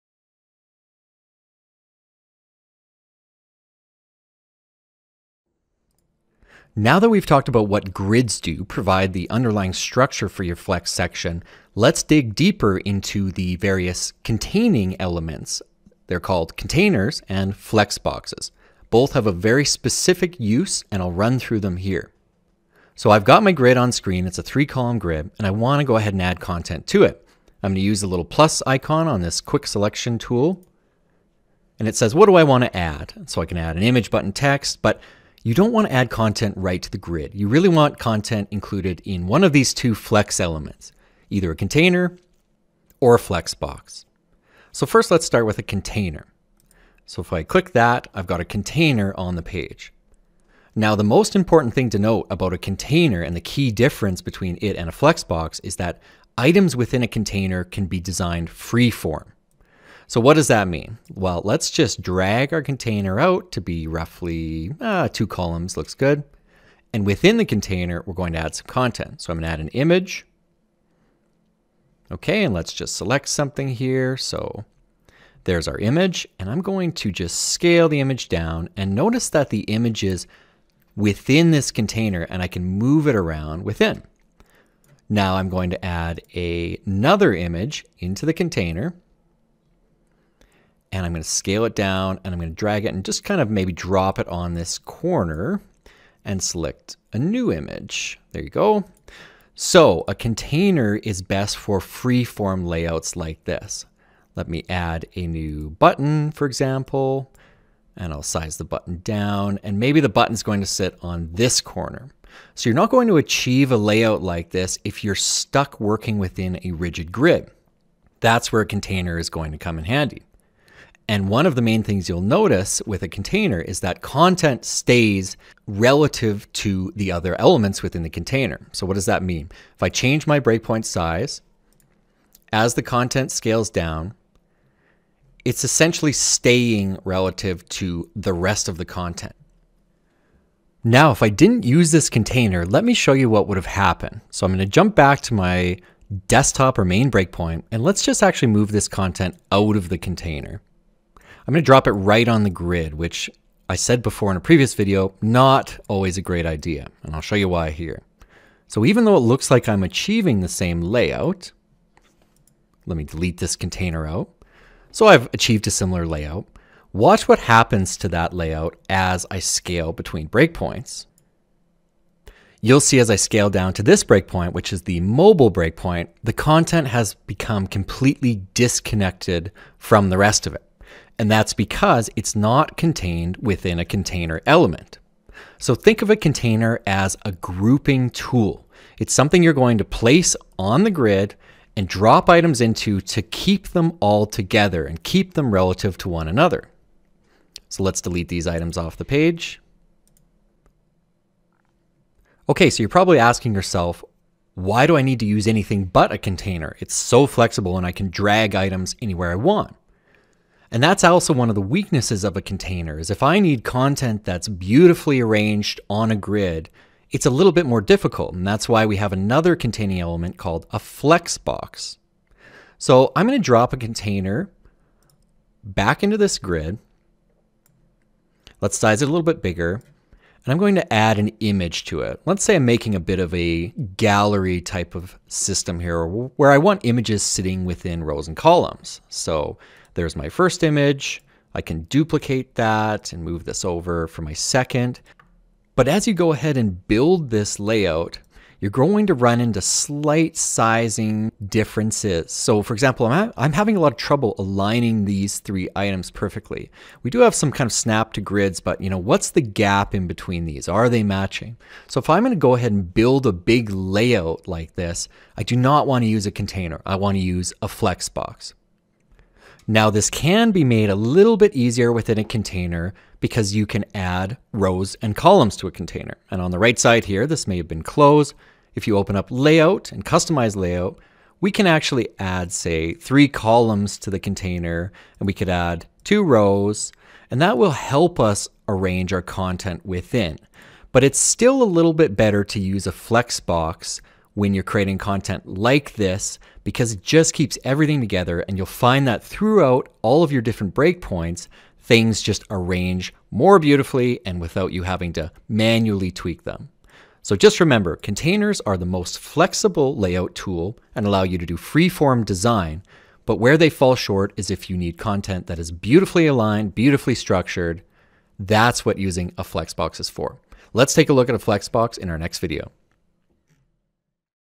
Containers | Essential Flex Training by Without Code
*Note: this training applies to Flex v1. Training for the new Flex is coming soon.

In episode 5 of our Flex Essentials training series, we take a look at containers. Flex is a free-form responsive design mode that’s included in the Without Code website builder.